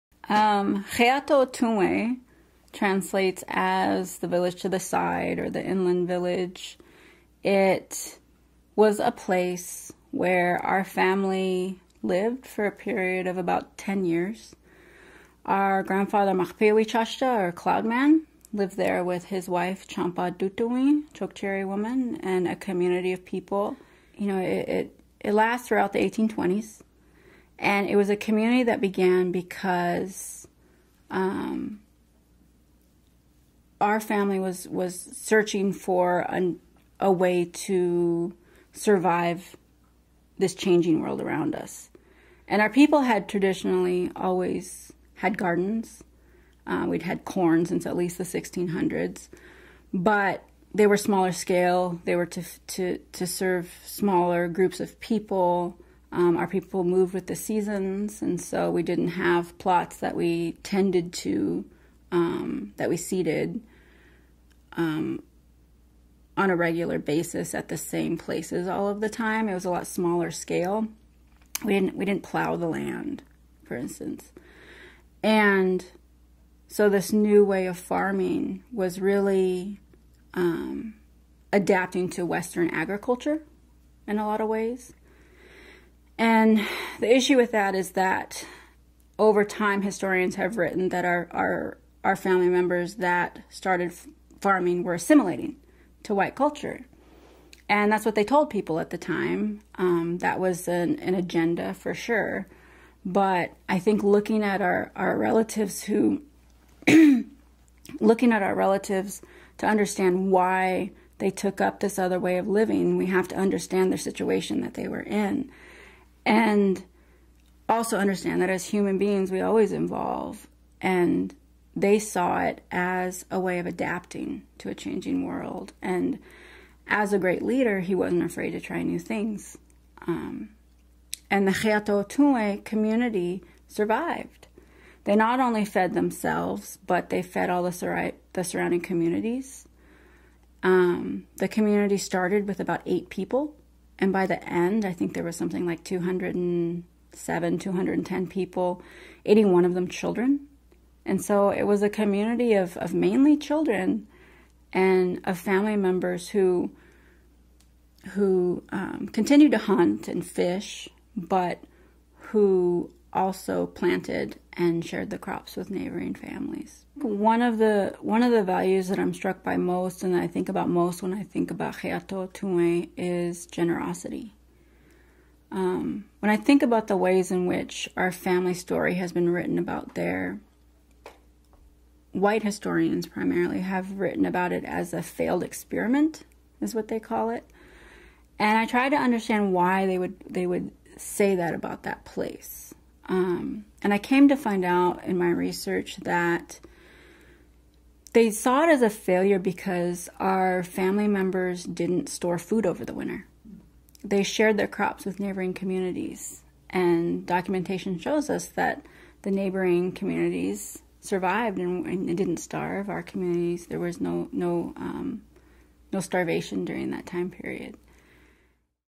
Hyeáta Othuŋwe Village Family Stories1
Kate Beane, Flandreau Santee Sioux, describes her family's history at Hyeáta Othuŋwe, a historic Dakota Village near the shores of Bde Maka Ska near where downtown Minneapolis is now. This video, by Mona Smith, is used with permission from Allies: media/art and is incorporated in the Confluence of Cultures module of the Big River Journey Online program